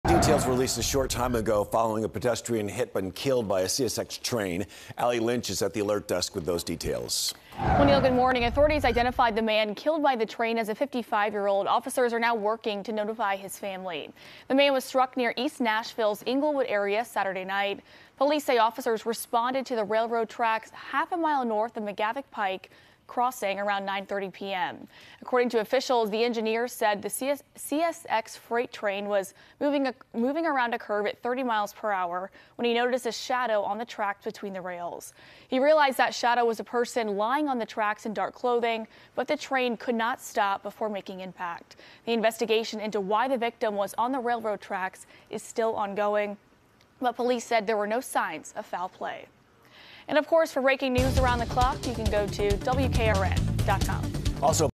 Man hit, killed by train in East Nashville
A man is dead following a collision involving a freight train near East Nashville’s Inglewood area Saturday night.